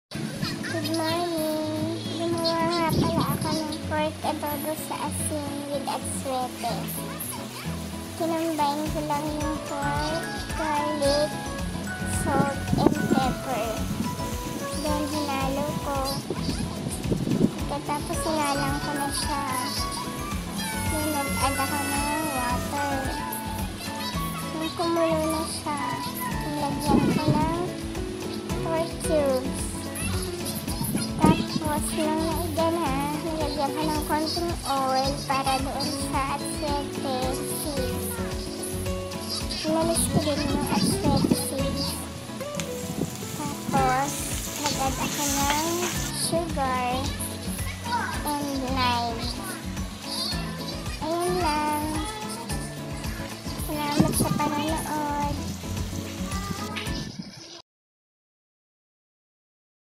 pork adobo sa asin with asuete
pork
garlic 1 head
salt
pepper
water
pork cube
oil
annatto seeds
sugar
lime juice